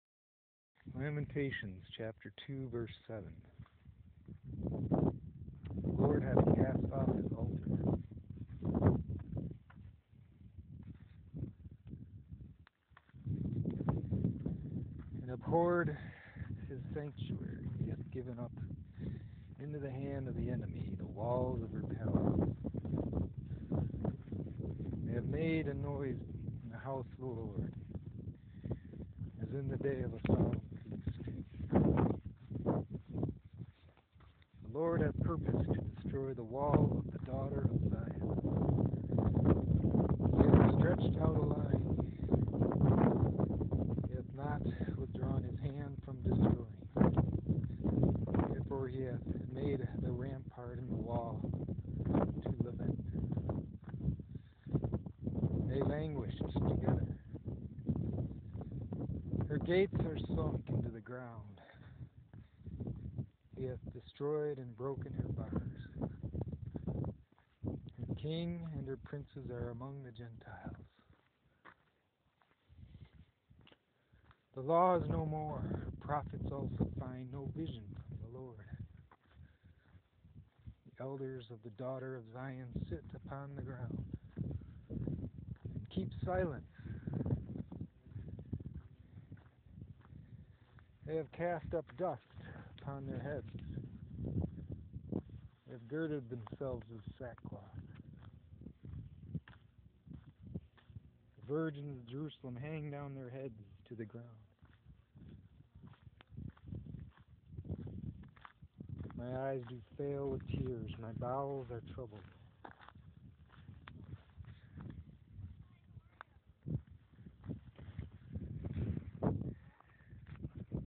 LAM 2 : MINE EYES DO FAIL WITH TEARS
The Lord hath cast off his altar, he hath abhorred his sanctuary, he hath given up into the hand of the enemy the walls of her palaces; they have made a noise in the house of the LORD, as in the day of a solemn feast.

8 The LORD hath purposed to destroy the wall of the daughter of Zion: he hath stretched out a line, he hath not withdrawn his hand from destroying: therefore he made the rampart and the wall to lament; they languished together.

9 Her gates are sunk into the ground; he hath destroyed and broken her bars: her king and her princes are among the Gentiles: the law is no more; her prophets also find no vision from the LORD.

10 The elders of the daughter of Zion sit upon the ground, and keep silence: they have cast up dust upon their heads; they have girded themselves with sackcloth: the virgins of Jerusalem hang down their heads to the ground.

11 Mine eyes do fail with tears, my bowels are troubled